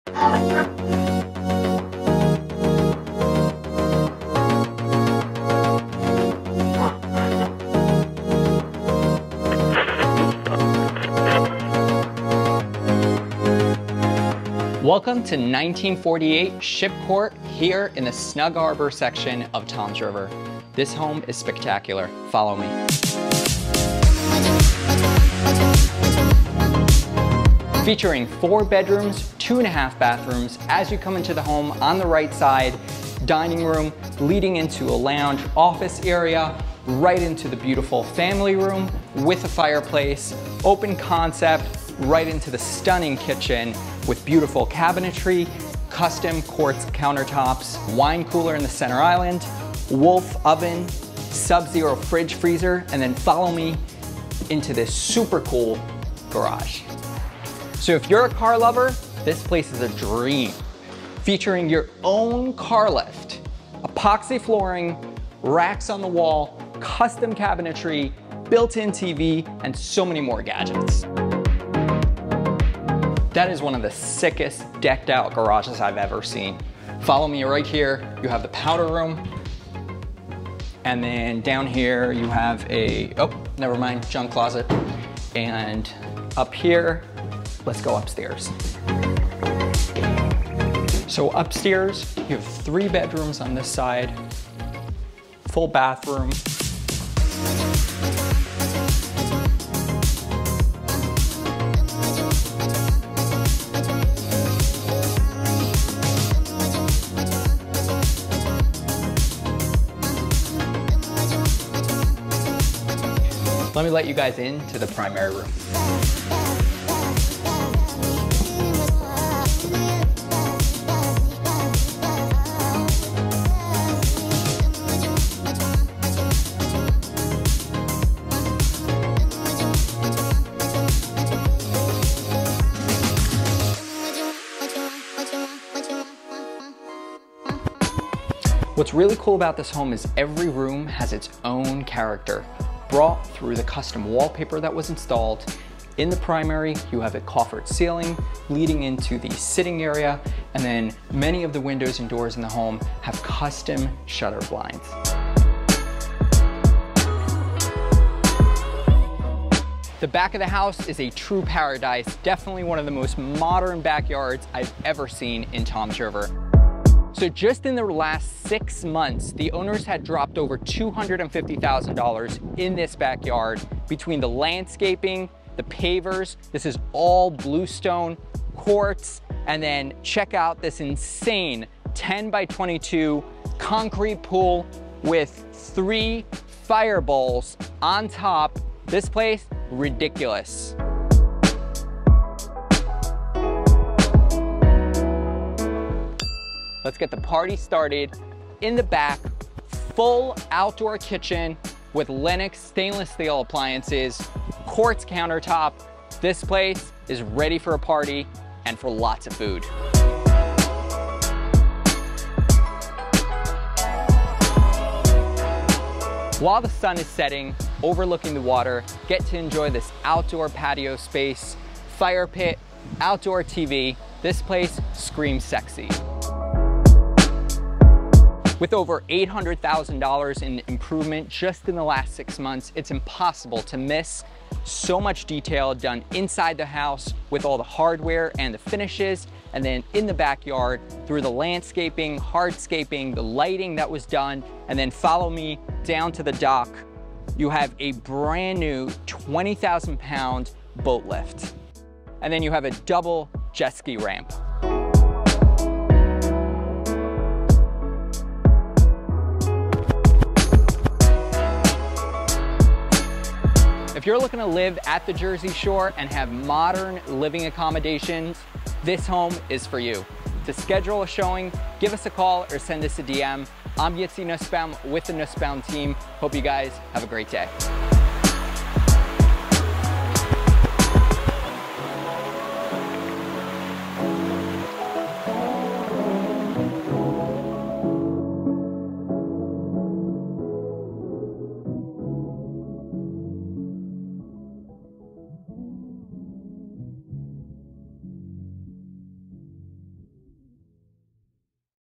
Million Dollar home in Snug Harbor Sold $50K Over Asking Price
SOLD IN SNUG HARBOR - TOMS RIVER

4 Bed | 2.5 Bath | 2,700Ft.²

SOLD $1,700.000

Snug Harbor never looked this luxurious! Welcome to our newest Waterfront listing featuring 4 beds 2.5 baths and a backyard ready for a party! Pulling up, you will instantly notice that the owners did not leave a single stone unturned when Upgrading this home to this modern luxury façade. Techo blue stone pavers & detailed professional landscaping lead up to the stucco-styled home. In the foyer, you will be met by the staircase leading upstairs, a radiant heated tiled floor & a dining room to the right. The dining room features a tray ceiling with a chandelier, large windows overlooking the front property, and crown molding that continue throughout the home.

Adjacent to the dining you will find an additional area perfect for a living extension or home office. The back living room offers a stone-faced fireplace with mantle, wood floors, recessed led spotlights, and large windows overlooking the backyard. Between the kitchen and living room sits the eat-in table along with french doors leading to the backyard. In the kitchen, you will find an oversized island, pot filler at the stove, under cabinet lighting, wine cooler, stunning quartz countertop, Sub-Zero & Wolf stainless steel appliances, and custom milled cabinets that offer tons of storage. Off the foyer, you will find an updated powder room before walking upstairs.

Upstairs and to the left will bring you to the primary suite that features sophisticated wallpaper, wood floors, crown moldings, tray ceiling, lounge area, and dual Juliet balconies, one overlooking the front yard and the other overlooking the foyer. The primary bath features a stand-up shower, a large tub, double sinks, modern light fixtures, and a large walk-in closet. You will find a full bathroom and three additional large bedrooms upstairs, 2 of which also include Juliet balconies overlooking the backyard pool area.

Back downstairs and in the backyard will bring you to the true heart of the home. Out back, you will find all you need for that much-deserved rest and relaxation. Featuring an inground pool with granite waterfall and three fire bowls, a full outdoor lynx kitchen, a fire pit, and an outdoor entertainment area with a TV. Watersports is more of your thing; you're in luck! This home includes a 20,000lb boat lift and a floating jet ski dock for two featured on the widest lagoon (100Ft Approx) in the area, making it a breeze to launch and dock your boat. This home truly embodies the vision of living at the Jersey Shore.

Other notable features of this home include a central vacuum, a brand new AC unit, Anderson 400 series windows with custom shutter blinds, a sun awning, a 2-car garage with epoxy floors, a wet bar, custom garage doors, a car lift, and wall storage. The use of weave technology on exterior walls adds proper airflow & is a 92% efficient home with two air zones and an independent zone for radiant flooring on First Floor Foyer & Kitchen. Modern living at the Jersey shore has never looked so sexy...

𝑻𝒉𝒆 𝑵𝒖𝒔𝒔𝒃𝒂𝒖𝒎 𝑻𝒆𝒂𝒎 🏡
𝗥𝗲𝗮𝗹 𝗘𝘀𝘁𝗮𝘁𝗲 𝗣𝗿𝗼𝗳𝗲𝘀𝘀𝗶𝗼𝗻𝗮𝗹𝘀
𝐨:𝟕𝟑𝟐-𝟑𝟒𝟏-𝟐𝟐𝟐𝟕
𝐜:𝟕𝟑𝟐-𝟗𝟎𝟖-𝟏𝟓𝟕𝟑
TheNussbaumTeam.com